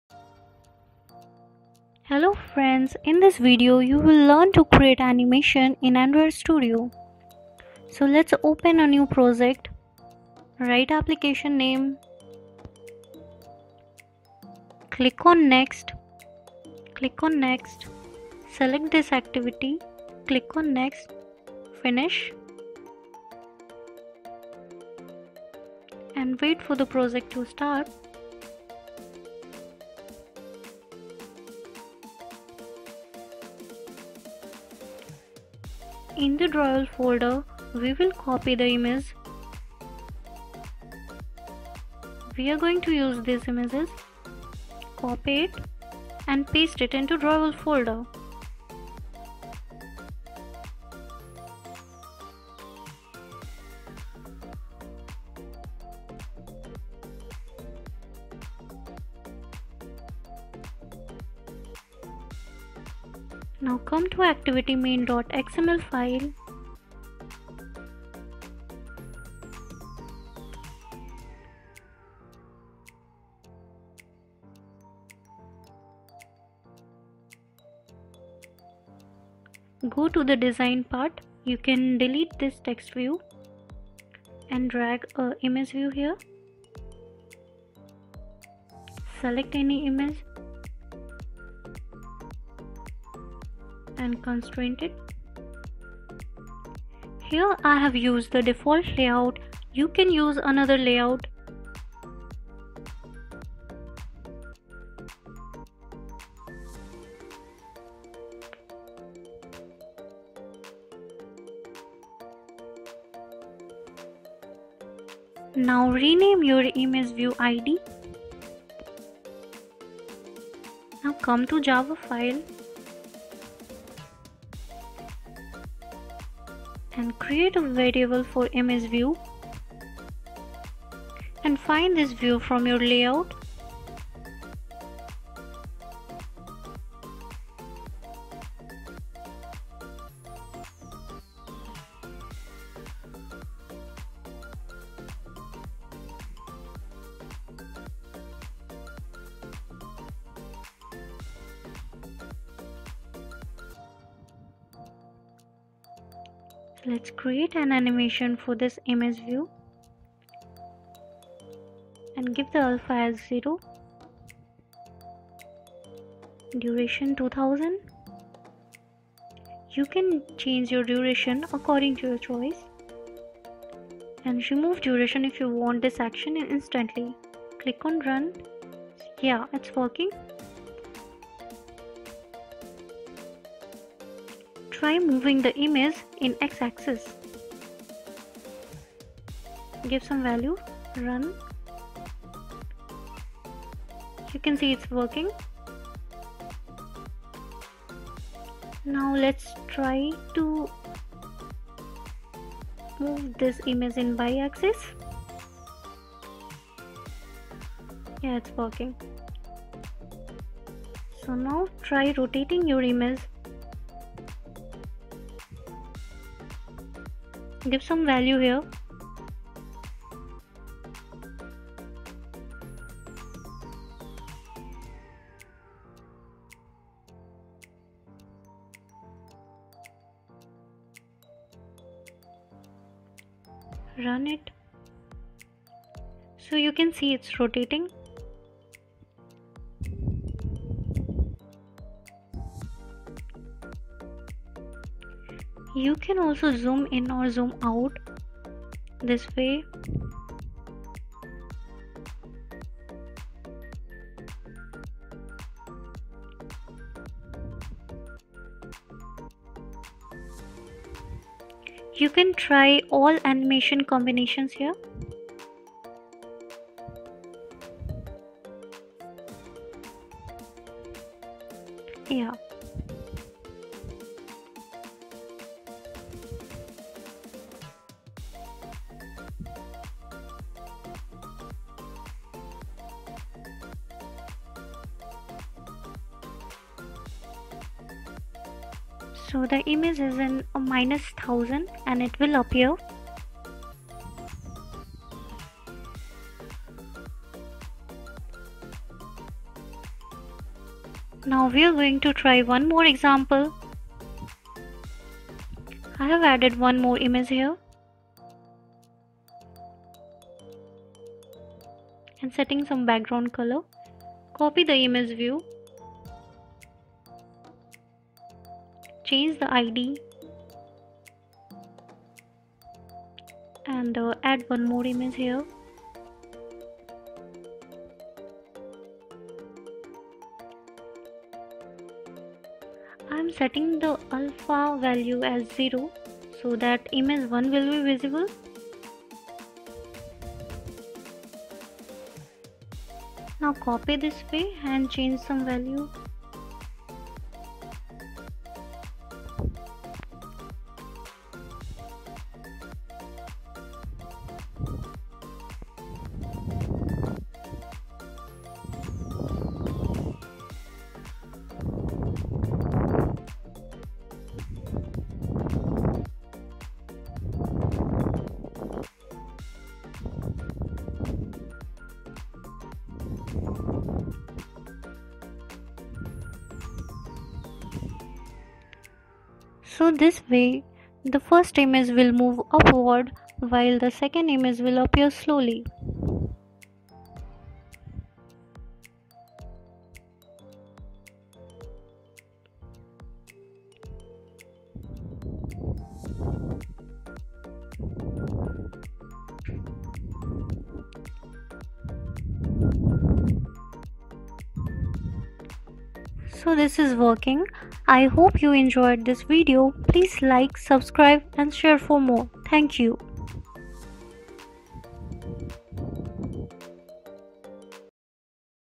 How To Add Animation In Android App | Android App Development Tutorial
Learn How To Add Animation In Your Android App Using Android Studio | Animate Any Image In Android App | Android App Development Tutorial

In this tutorial i'll describe how to do basic animation using Android Studio. We'll be adding a number of animations in it. Finally you'll be able to create your own animation by combining all the animations.

Basically, You'll see the transition of image on X axis And Y axis, Scaling of images, Image fading animation, Image popup animation, Image change animation and more.

This Was a short tutorial on Adding Animation into your Android App.